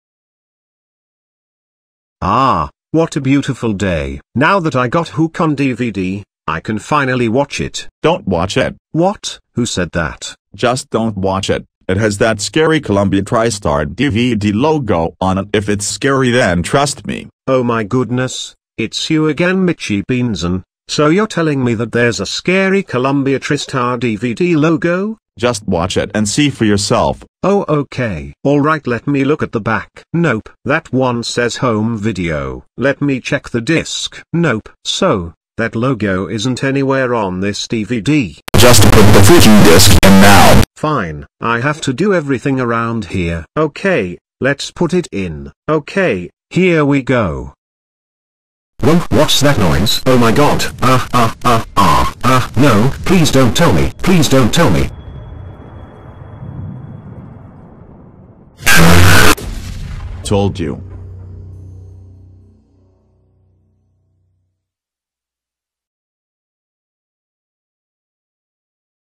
Belinea Computer Watches The Columbia Tristar DVD Logo (Remake)
For Hope Ture Video 9600 Cuddles
I used readloud.net for the voices.
Belinea Computer - Brian
Mitchy Beanson - Eric